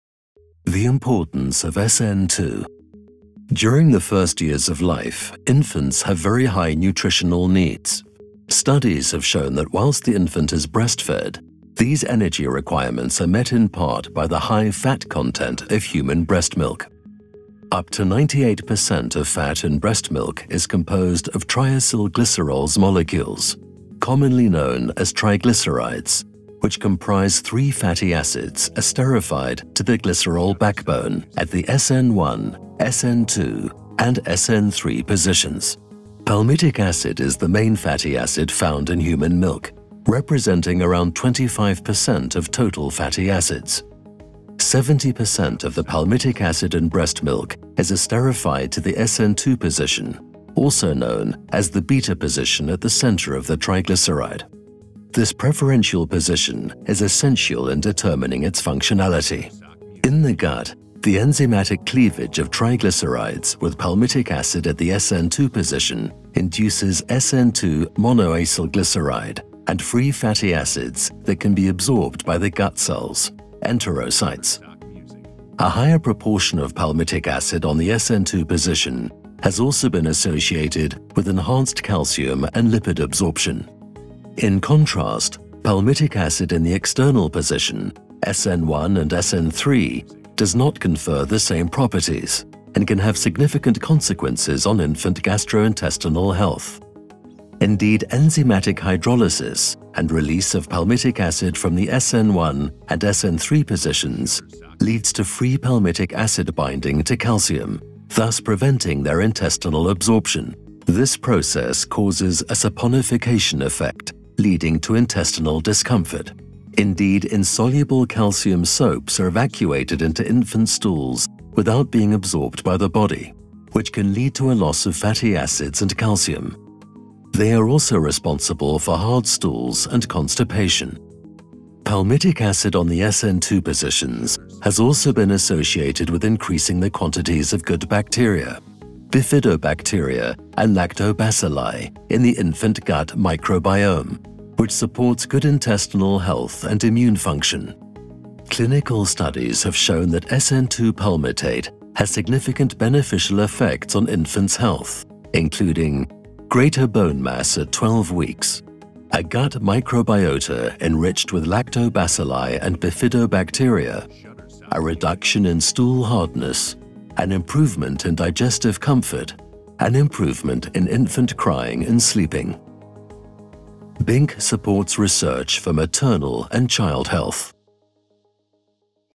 Science Animation - Fatty Acids in the SN2 Position
Fat content in breast milk provides around 50% of the energy necessary for newborns’ growth and development. 98% of fat in breastmilk is composed of triglycerides, which contain 3 fatty acids bound to a glycerol backbone. These fatty acids consist of SN1 and SN3 located in the external positions of the triglyceride, and SN2 at its center. Palmitic Acid is the main fatty acid found in human milk and 70% is esterified at the SN2 position. Find out why Palmitic Acid’s esterification at the SN2 position is essential for its positive contribution to infants’ gastrointestinal health.